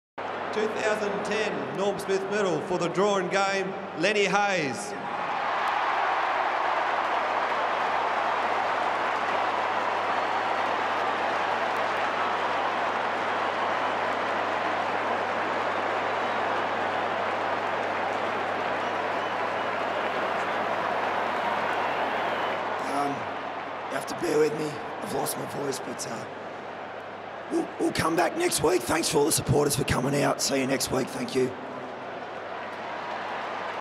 St Kilda vs Collingwood 2010 Grandfinal Norm Smith Medal
St Kilda vs Collingwood 2010 Grandfinal Draw St kilda's Lenny hayes wins the norm smith medal